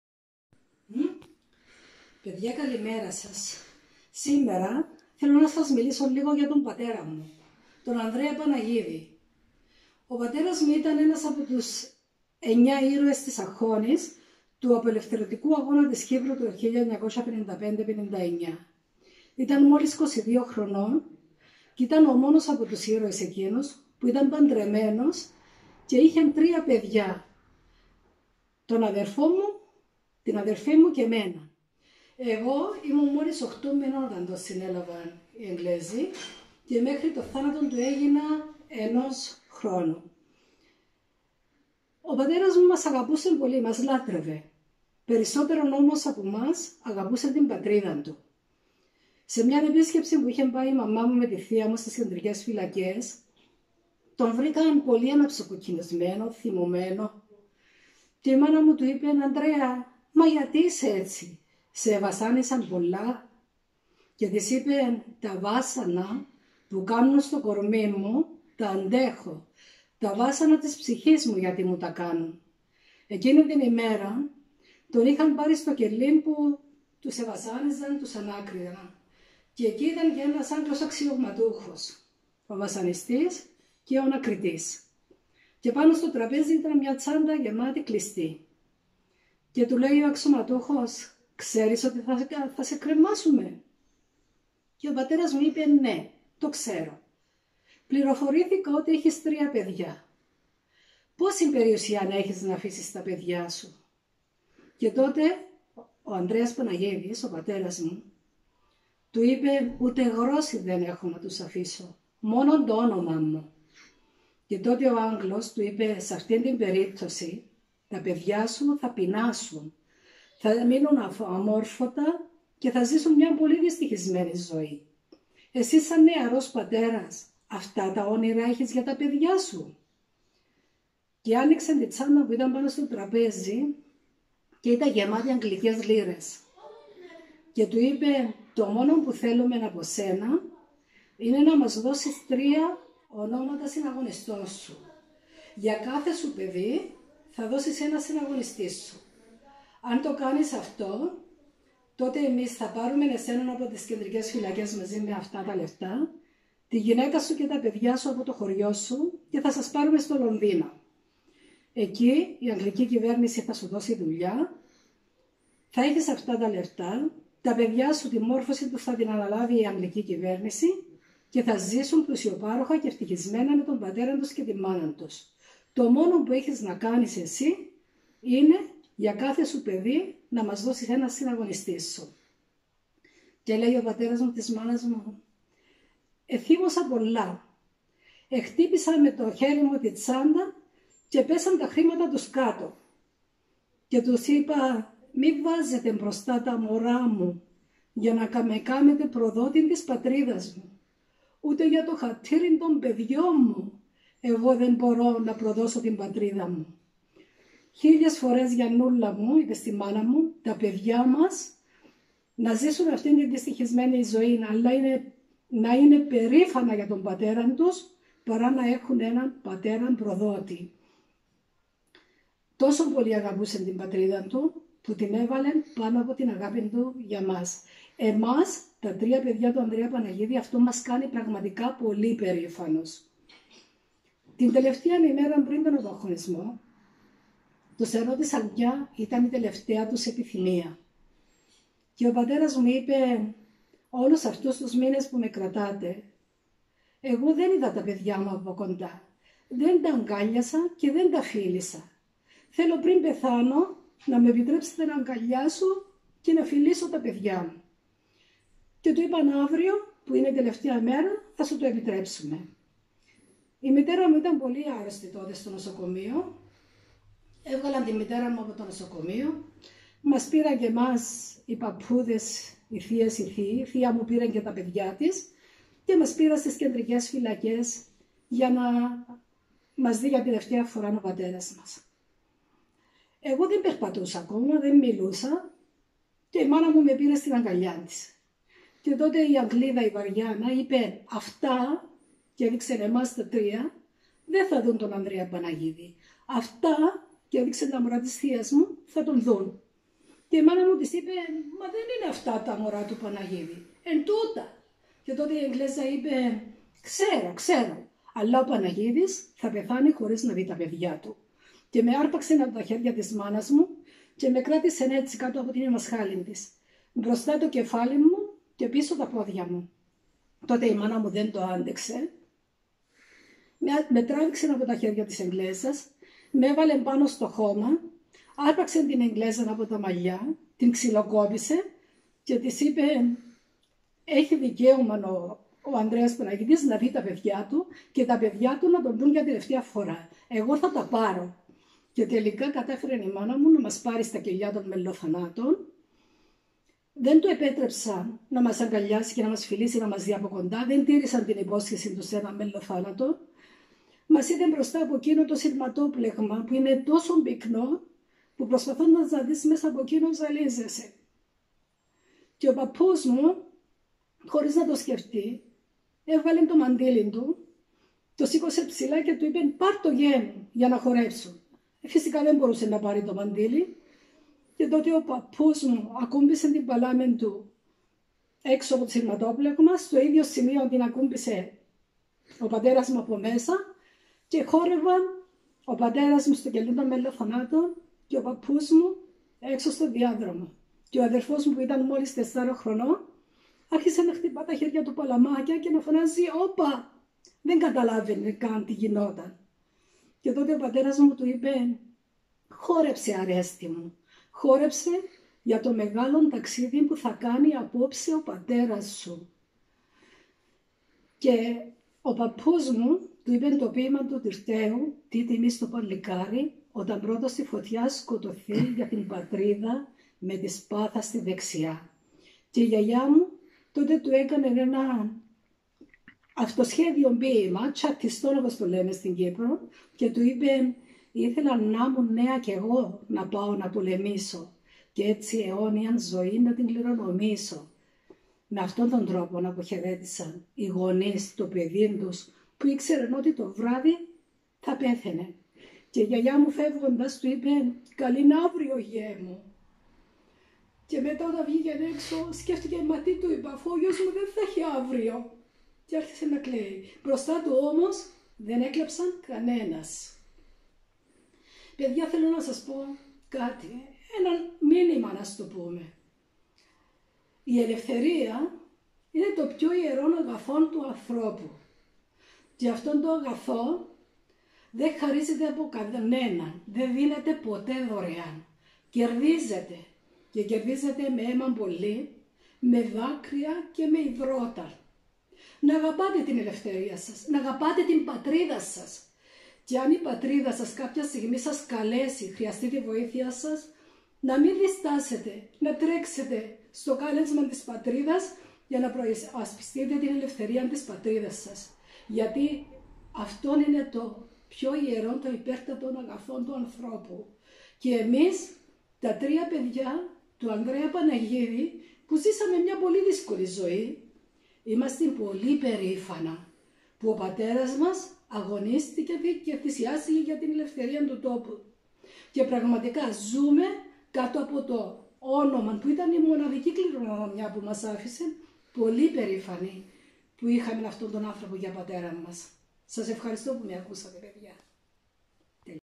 κ. Αυγή Παναγίδη κόρη ήρωος Ανδρέα Παναγίδη
Μιλάει στα παιδιά για τον ηρωα πατέρα της